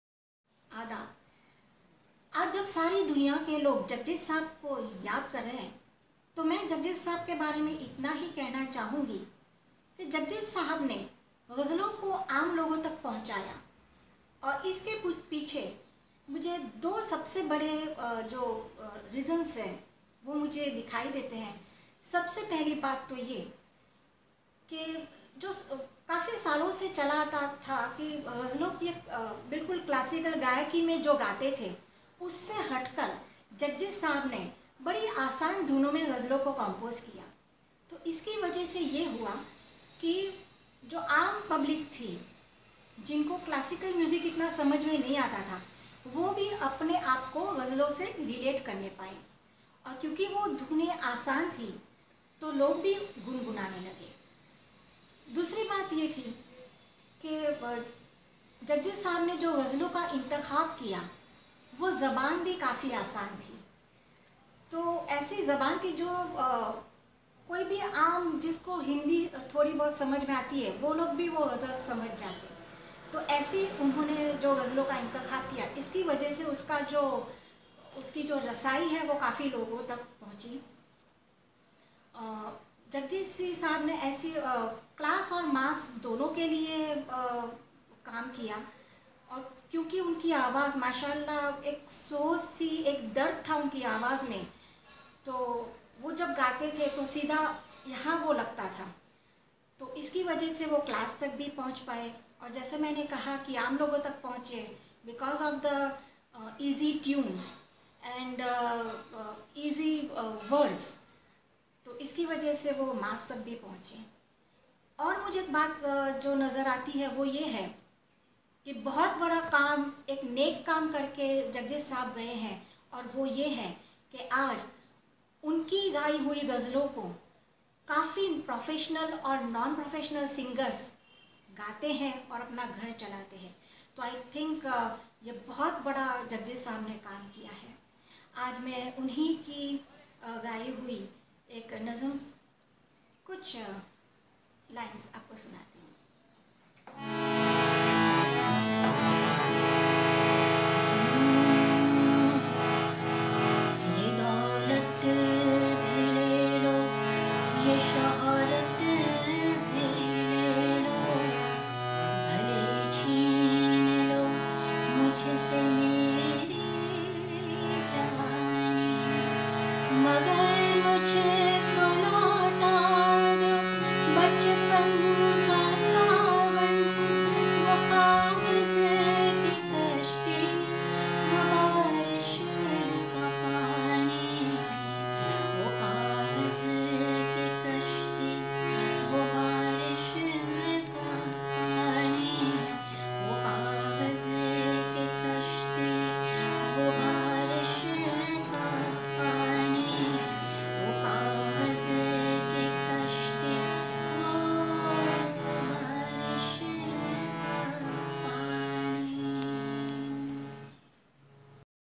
Sufi Singer Sonal Shah
Sufi Singer Sonal Shah pays tribute to Jagjit ji on his 6th death anniversary on 10th October 2017 at Ghazalsara.